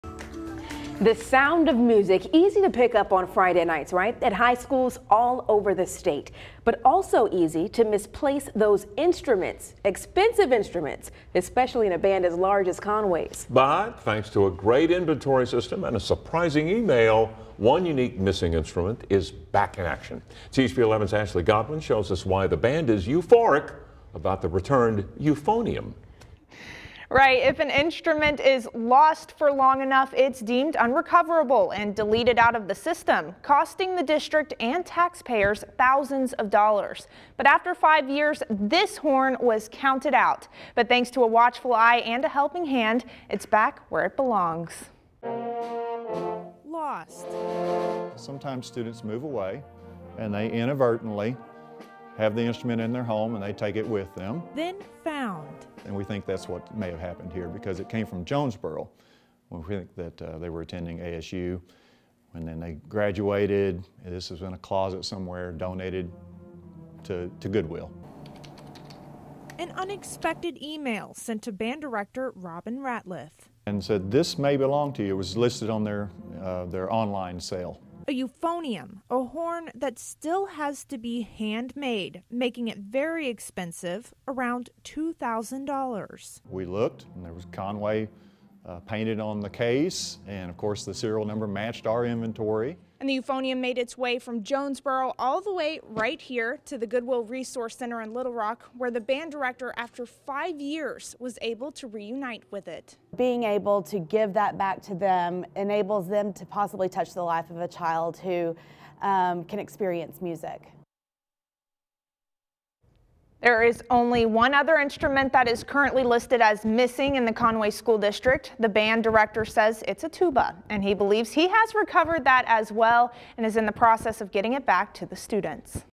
Conway school finds long lost instrument after five years
It's easy to misplace instruments, especially when the band is large. But thanks to a great inventory system and a surprising email, one missing instrument is back.